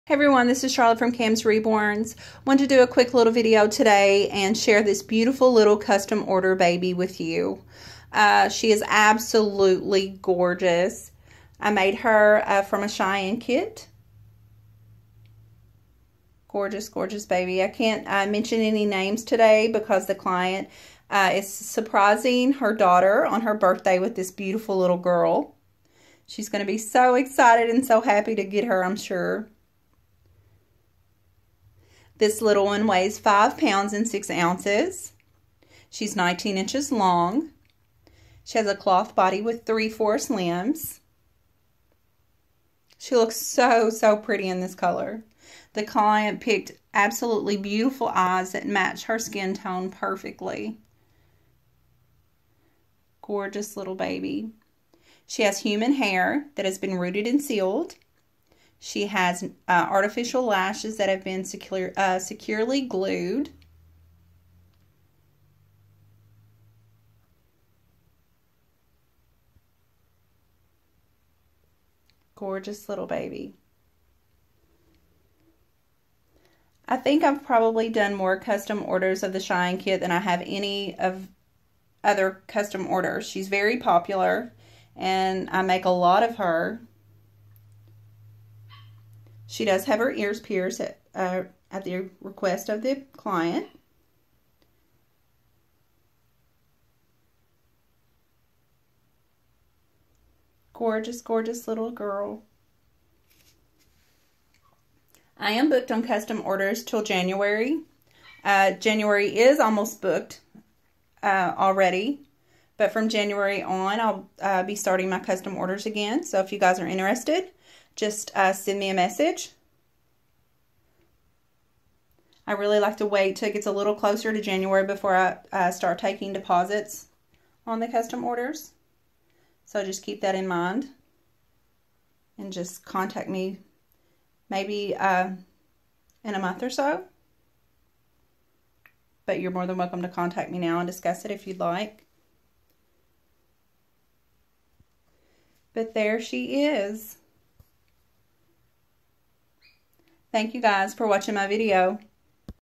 REBORN BABY GIRL IS STARTING HER JOURNEY HOME MONDAY 😊
CAMS REBORNS WOULD  LIKE TO WISH THIS LITTLE ONE A SAFE JOURNEY HOME MONDAY.She is a custom order and will be a birthday surprise so no names will be added to the post.  😊😊❤❤❤